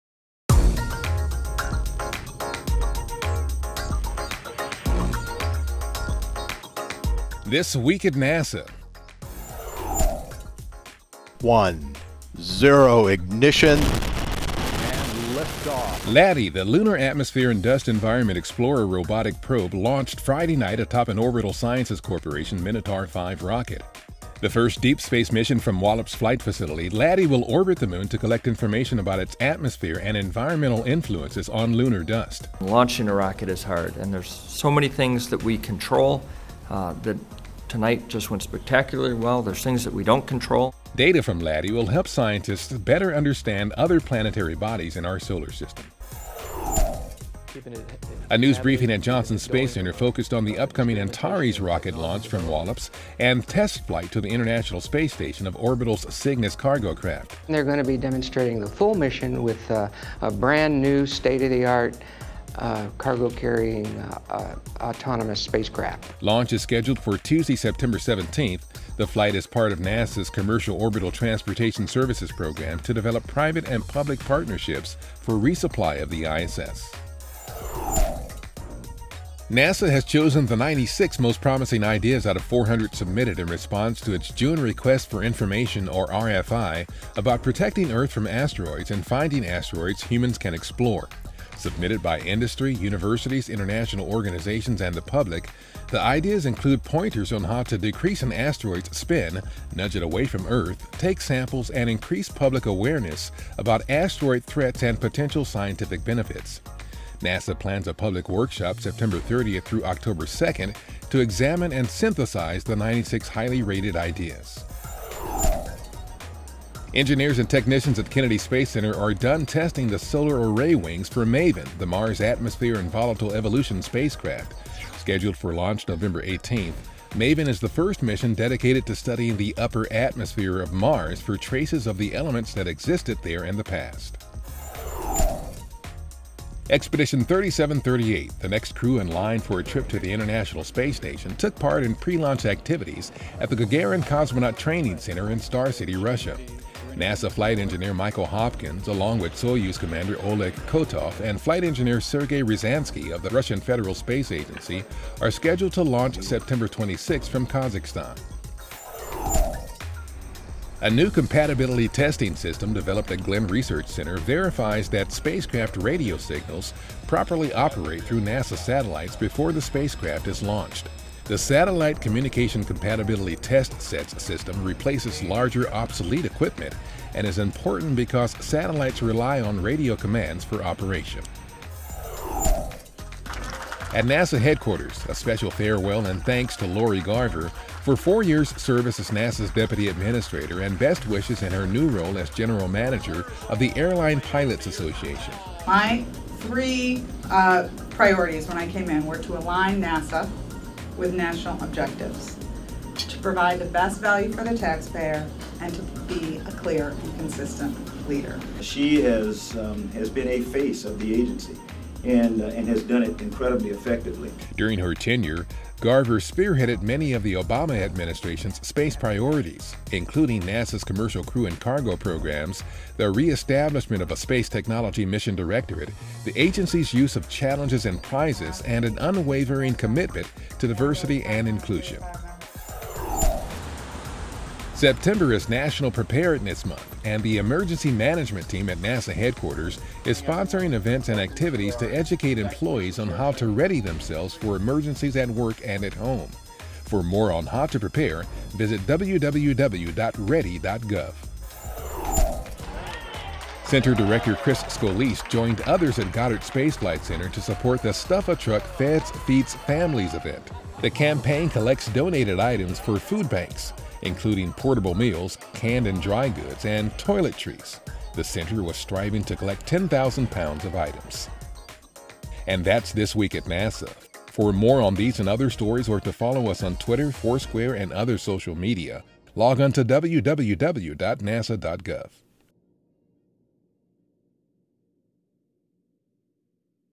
LADEE Launches! On This Week
LADEE, the Lunar Atmosphere and Dust Environment Explorer robotic probe launched Friday night atop an Orbital Sciences Corporation Minotaur V rocket. The first deep space mission from Wallops Flight Facility, LADEE will orbit the moon to collect information about its atmosphere and environmental influences on lunar dust. Data from LADEE will help scientists better understand other planetary bodies in our solar system. Also, Antares Update, Asteroid Ideas Selected, MAVEN's Wings, Next ISS Crew, Testing, Testing!, Lori Garver Farewell, Be Prepared! and more!